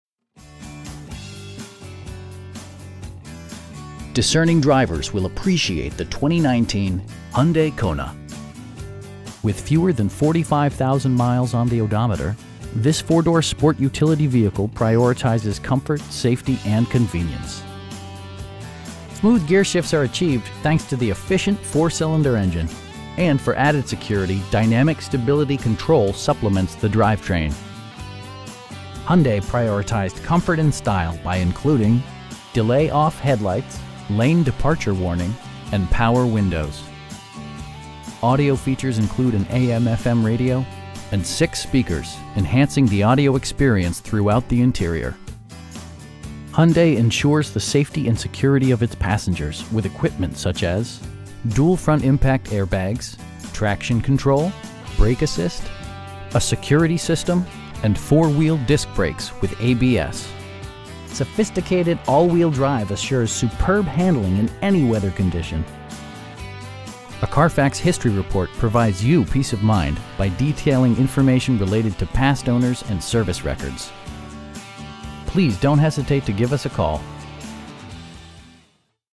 2019 Hyundai Kona SE in Danbury, CT 06810-5014
Discerning drivers will appreciate the 2019 Hyundai Kona. With fewer than 45,000 miles on the odometer, this 4 door sport utility vehicle prioritizes comfort, safety and convenience. Smooth gearshifts are achieved thanks to the efficient 4 cylinder engine, and for added security, dynamic Stability Control supplements the drivetrain. Hyundai prioritized comfort and style by including: delay-off headlights, 1-touch window functionality, a trip computer, power door mirrors, lane departure warning, remote keyless entry, and power windows. Audio features include an AM/FM radio, steering wheel mounted audio controls, and 6 speakers, enhancing the audio experience throughout the interior. Hyundai ensures the safety and security of its passengers with equipment such as: dual front impact airbags, head curtain airbags, traction control, brake assist, a security system, and 4 wheel disc brakes with ABS. Sophisticated all wheel drive assures superb handling in any weather condition. A Carfax history report provides you peace of mind by detailing information related to past owners and service records. We pride ourselves in the quality that we offer on all of our vehicles. Please don't hesitate to give us a call.